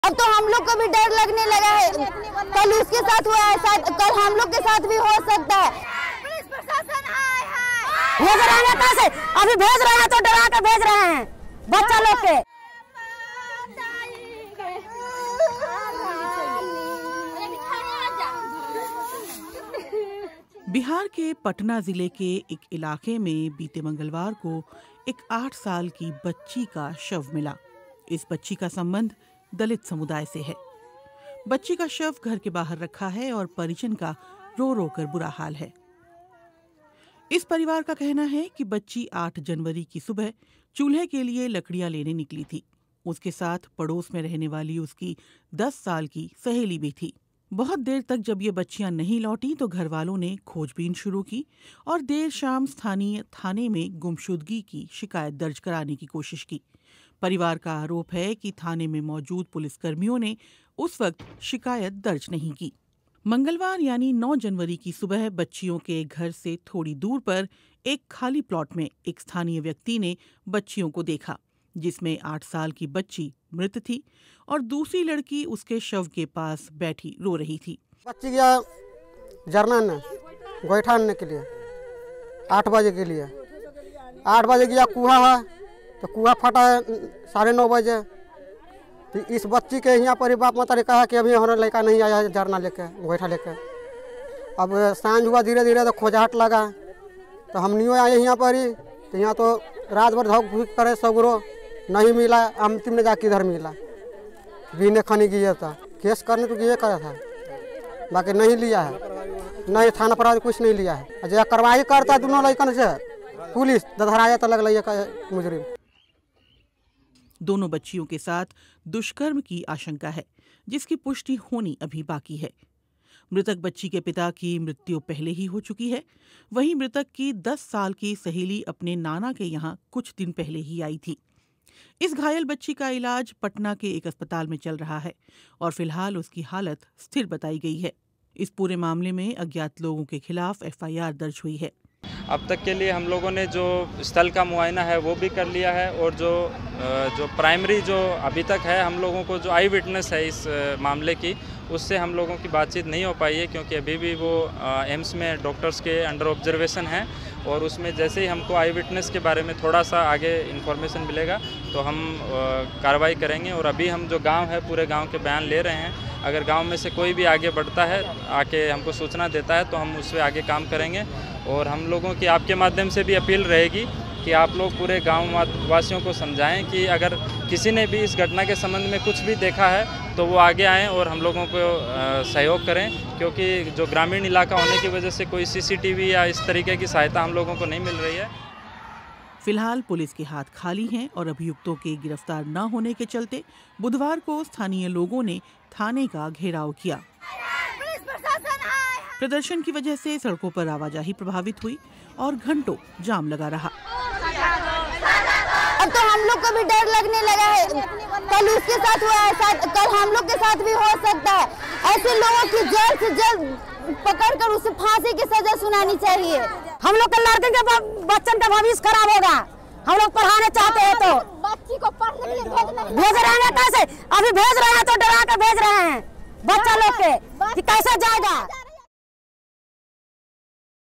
Bihar में Dalit बच्ची की मौत के मामले में अब तक क्या कुछ पता है? (BBC Hindi)
बिहार में पटना ज़िले के एक इलाके में बीते मंगलवार को एक आठ साल की बच्ची की लाश मिली. इस बच्ची का संबंध दलित समुदाय से है. बच्ची का शव घर के बाहर रखा है और परिजन का रो-रोकर बुरा हाल है. इस घटना से प्रांत में लोगों में आक्रोश है.

देखिए, सीटू तिवारी की ये रिपोर्ट